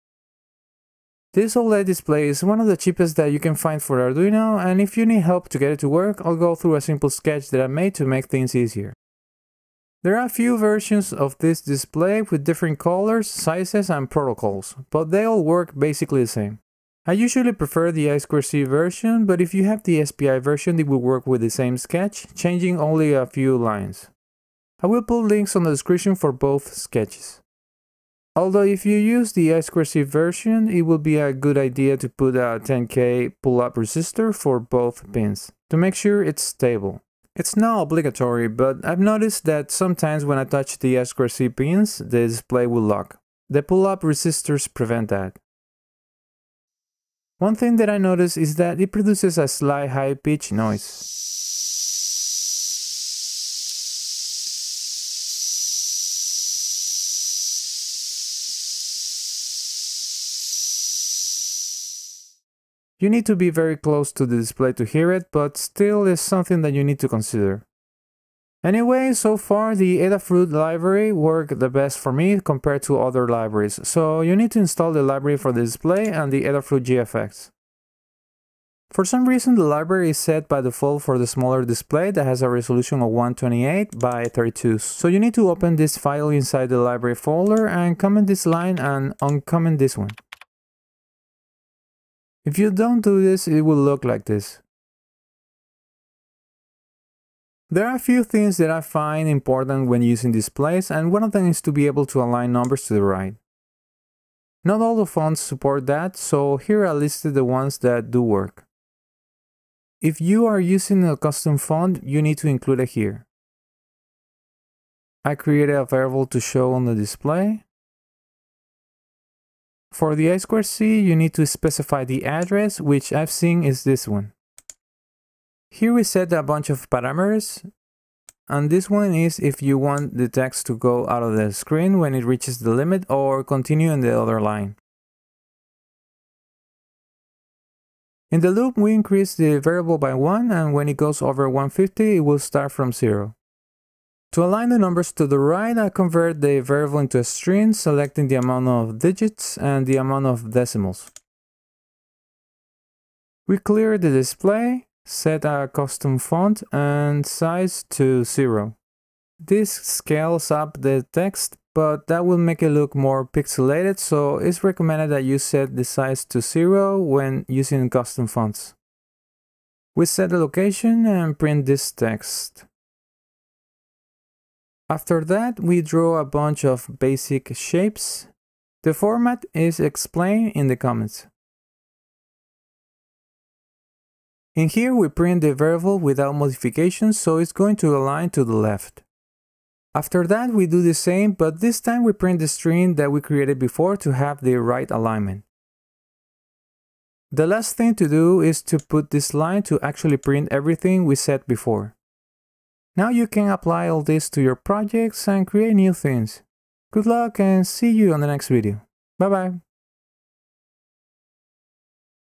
Arduino Tutorial: OLED 0.96" I2C/SPI Display
On this tutorial I show what you need to do to make the 0.96" 128x64 OLED display work with Arduino. This tutorial also works for the 0.91" 128x32 and the 1.3" 128x64 versions.

Recommended Tools:
- Electronic Parts Tester (ESR LCR-T4)
- Breadboard (good quality)
- Storage Case for Jumper Wire
- Multimeter (Fluke 17B+)
- Rigol DS1054Z Digital Oscilloscope